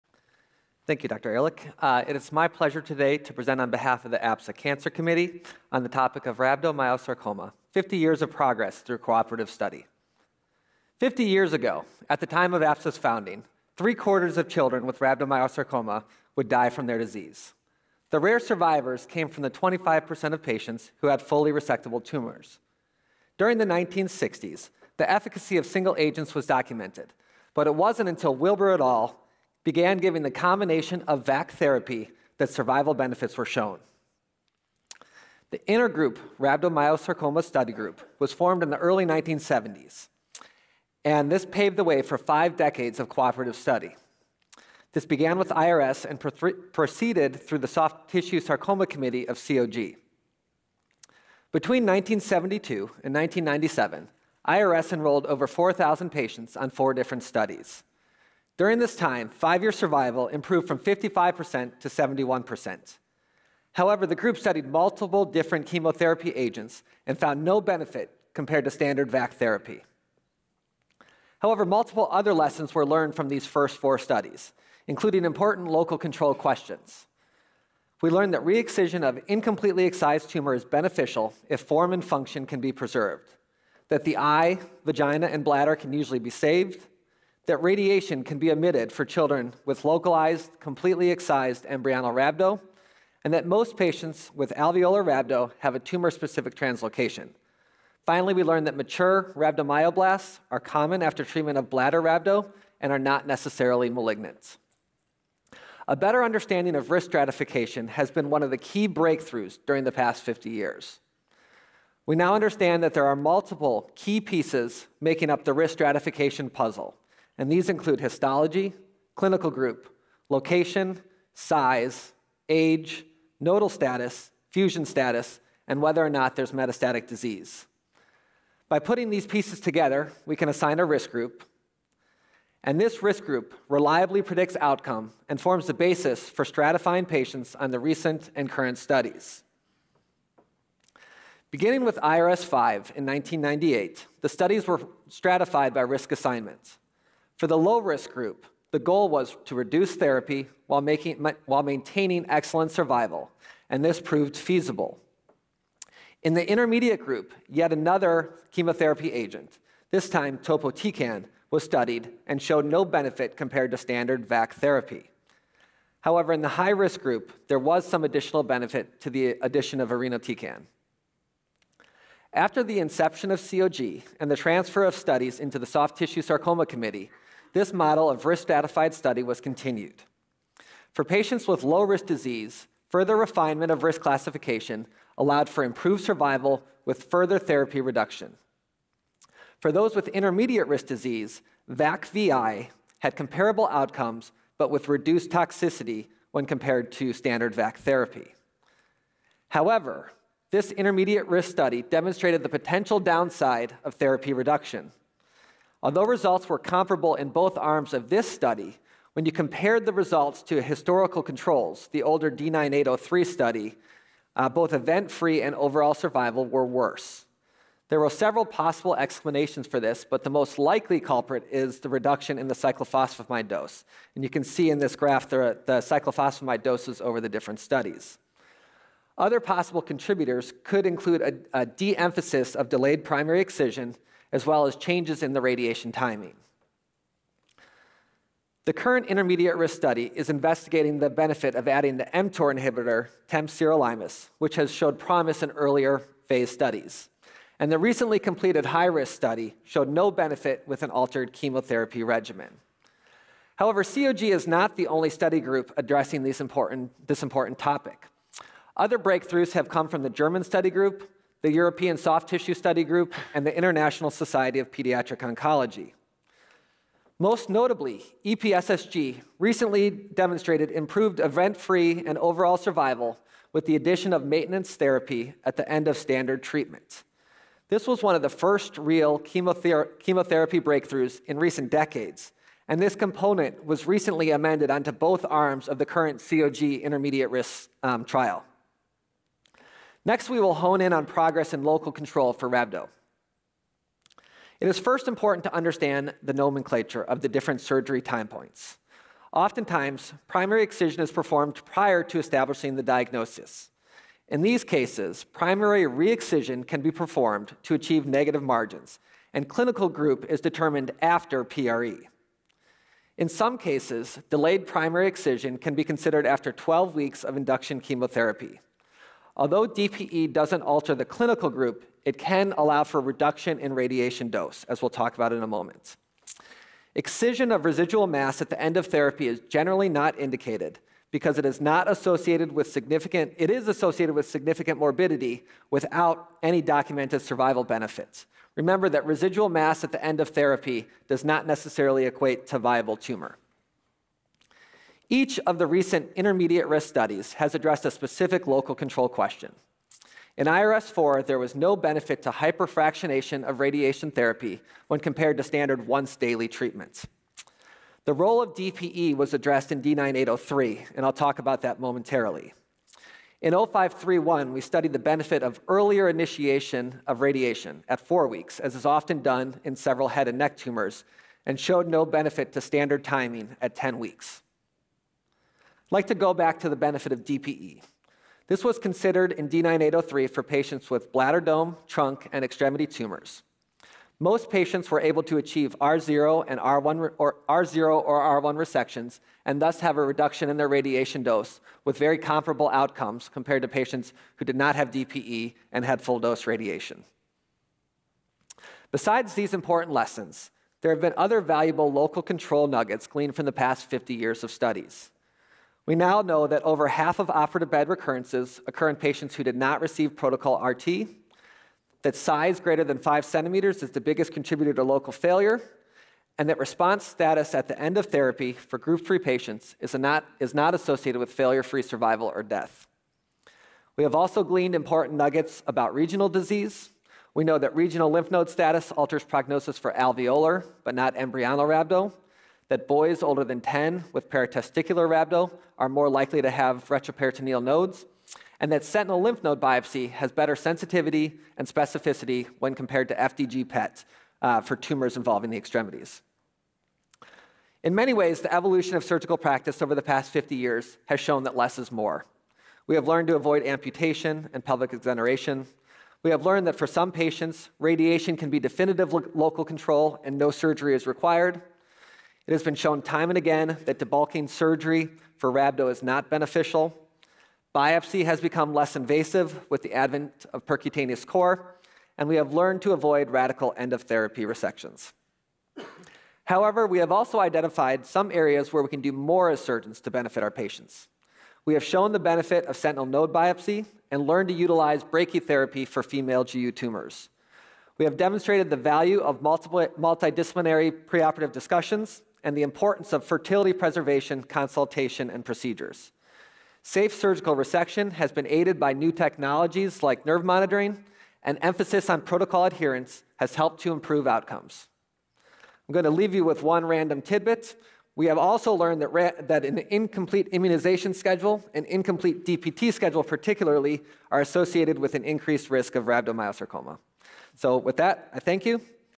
RMS: Rhabdomyosarcoma progress 2019
Timothy Lautz presents on the progress in surgical treatment of rhabdomyosarcoma as part of the Cancer committee session at the APSA 2019 annual meeting.